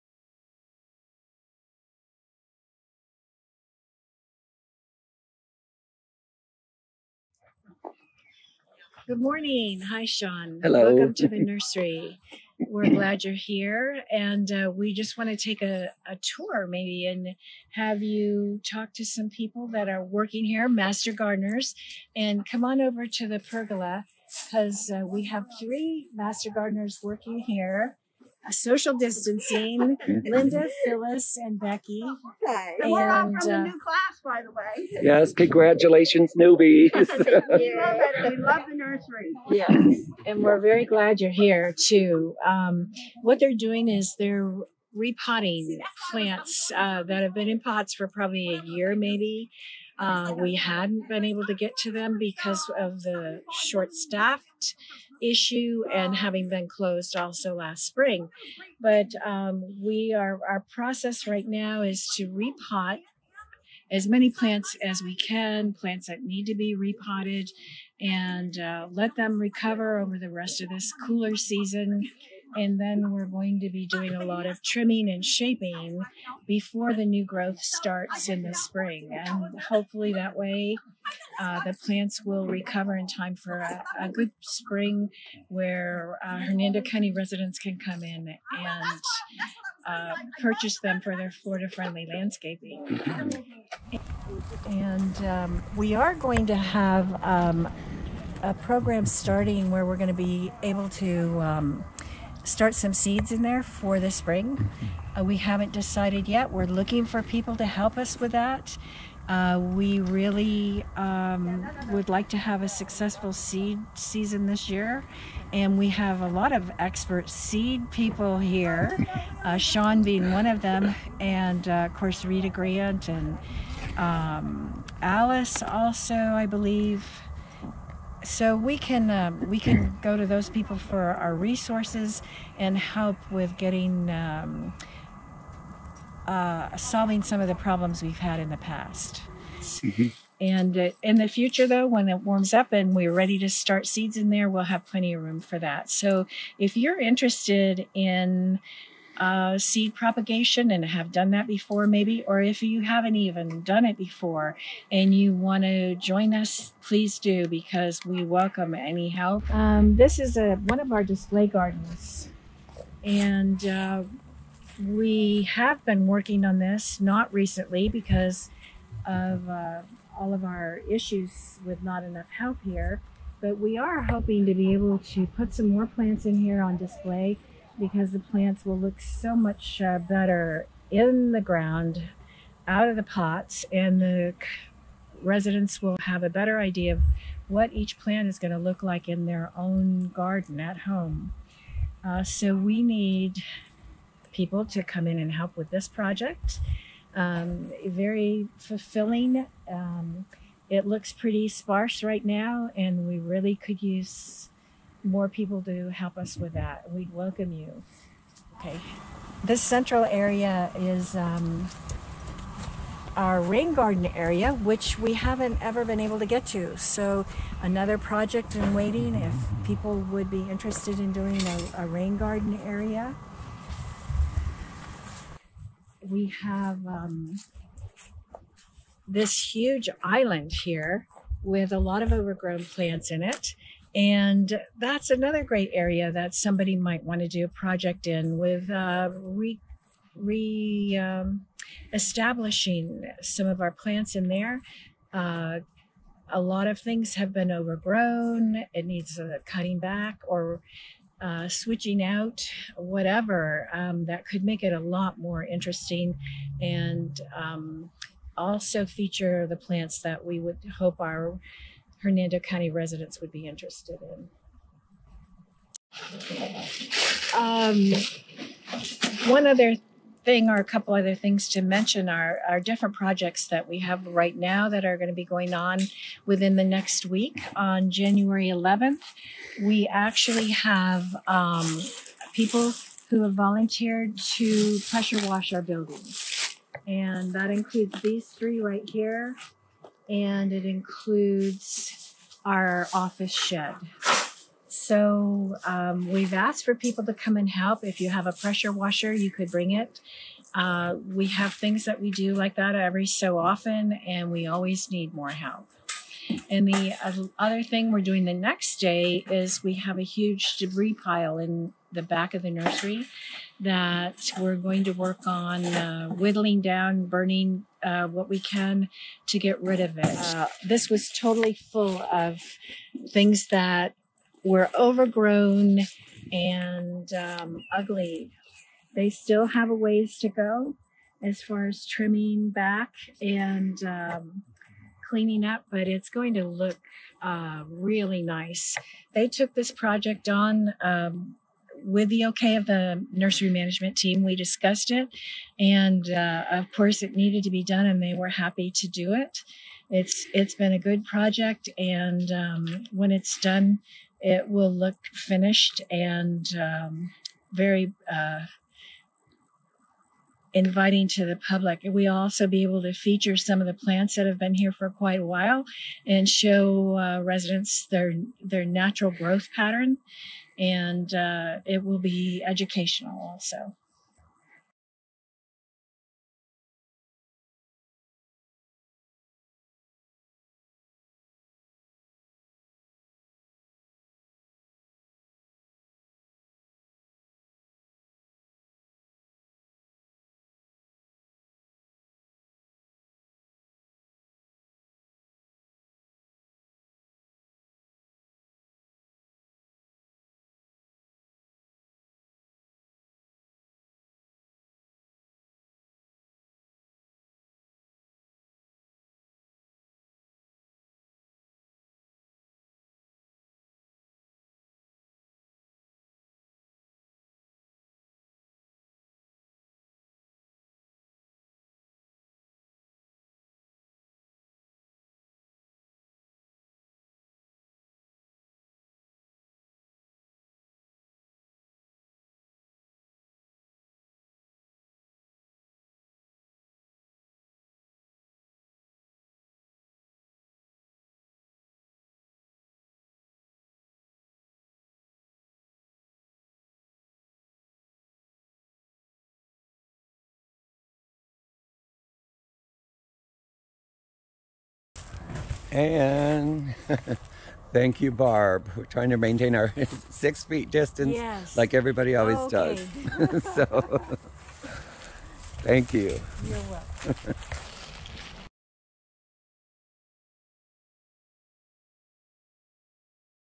Master Gardner Nursery News update with Barb Hart
This video is an update of January and February 2021 Projects at  the Hernando Master Gardener Nursery. These are presented to show the numerous volunteer opportunities available.  Please consider volunteering to complete these in time for our Hernando County residents this Spring. Check with the Nursery Management Team and VMS for opportunities to assist.